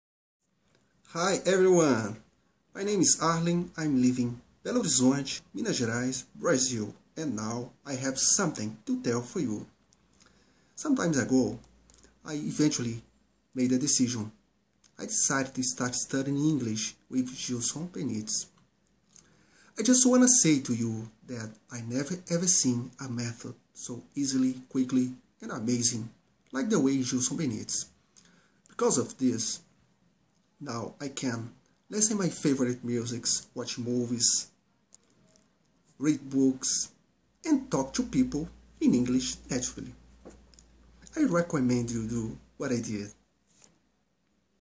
Aluno Arlem - Fluente em 2 anos
Meu aluno Arlem sempre foi muito esforçado e extrovertido, eu o conheci quando visitei uma faculdade em Belo Horizonte e fiz uma pequena palestra sobre o meu curso de inglês online e presencial. O Arlem se interessou e logo se matriculou. Ele foi aprendendo rápido e foi melhorando a pronúncia e chegou a fluência em 2 anos que é o tempo médio em que um aluno comprometido chega a fluência.